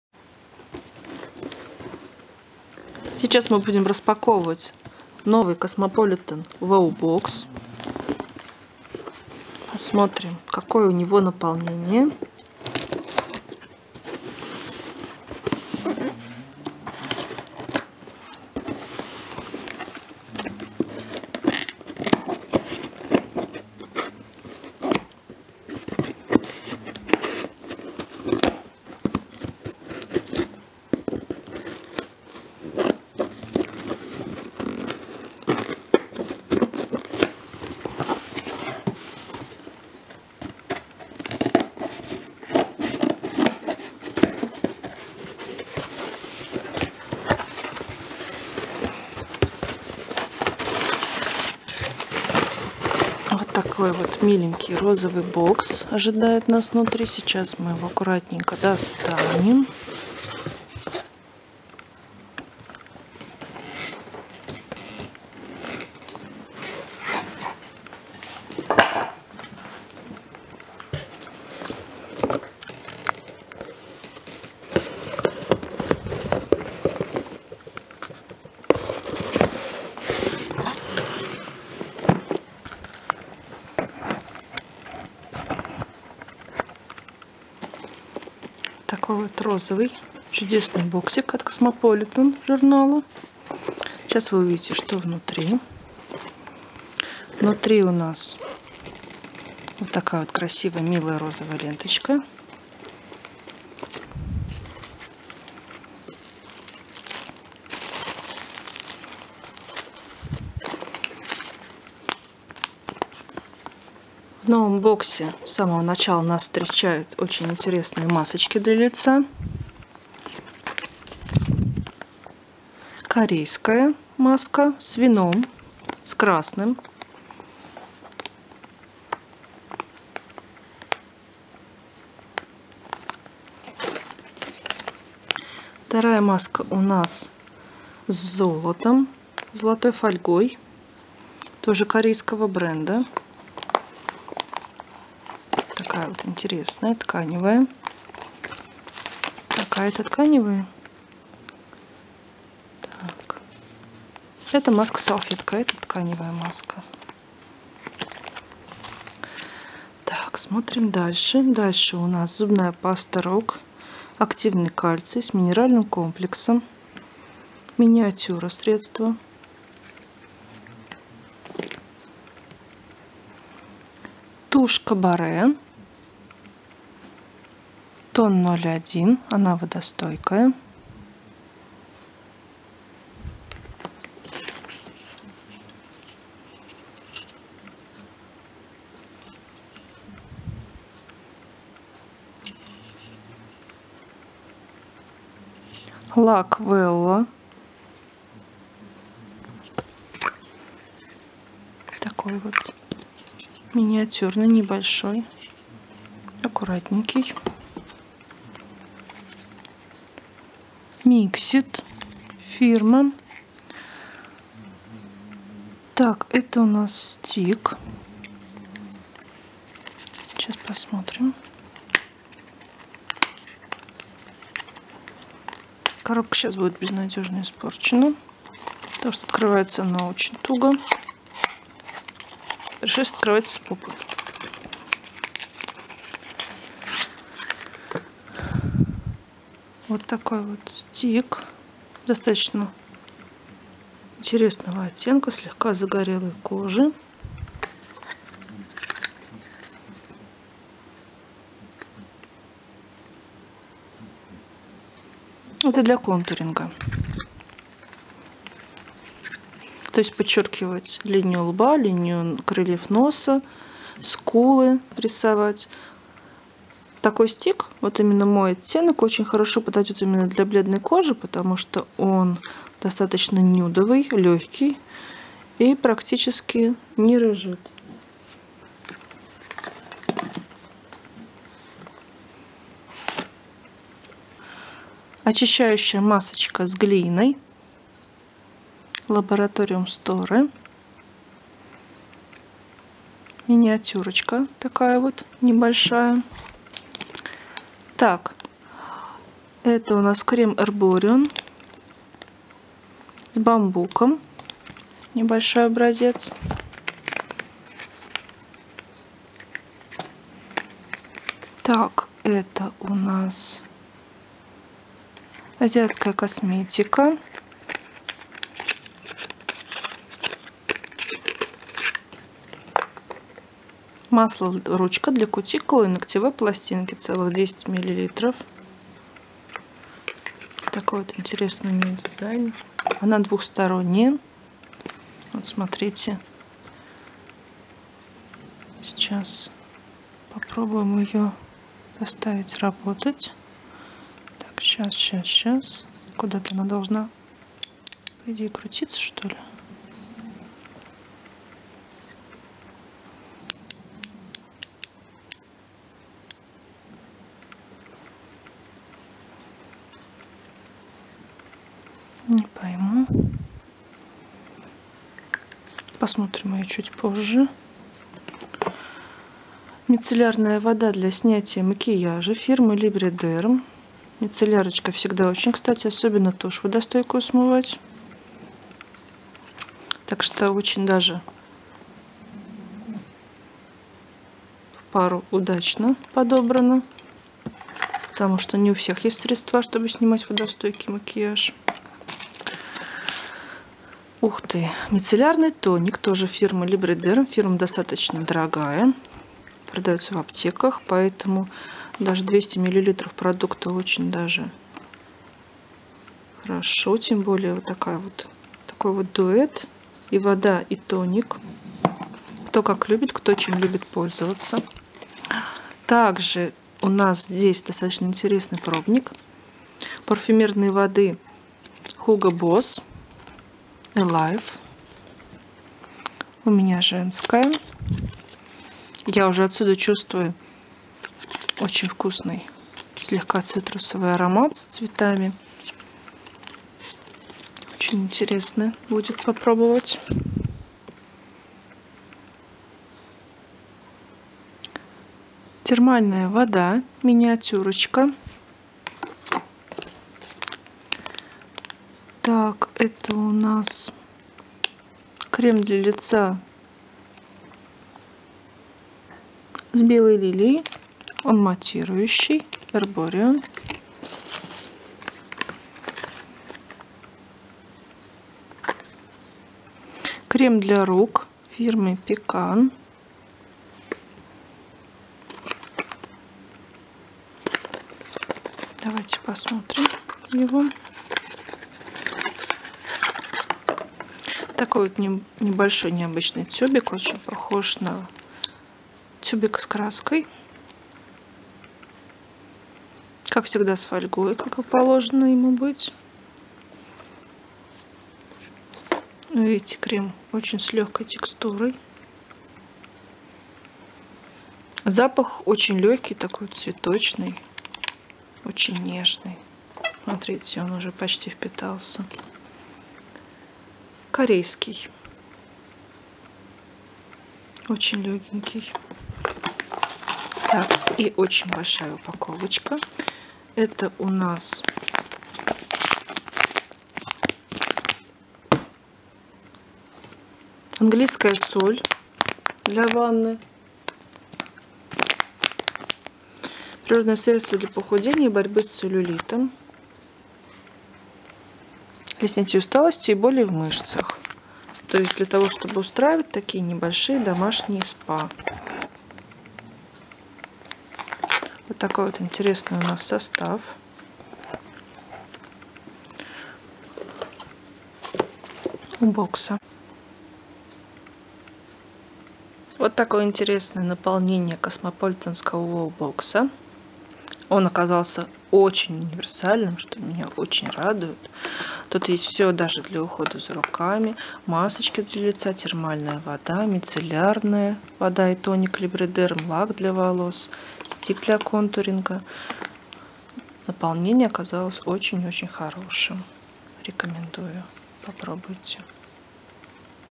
Коробочка красоты от Космополитан
Распаковка посылки.
Коробочка красоты WOW Box от Cosmopolitan!
Распаковка бокса WOW! Queen Box
Бокс, в котором есть всё: макияж, уход, стайлинг и парфюм.
- внутри целых 16 средств: 10 полноразмеров и 6 миниатюр
- lifestyle и professional бренды
- в каждом боксе тональный стик от Mixit, увлажняющая сыворотка от Uriage, мицеллярная вода и мицеллярный тоник от Librederm и еще 12 wow средств!
- ценность бокса 5200 руб.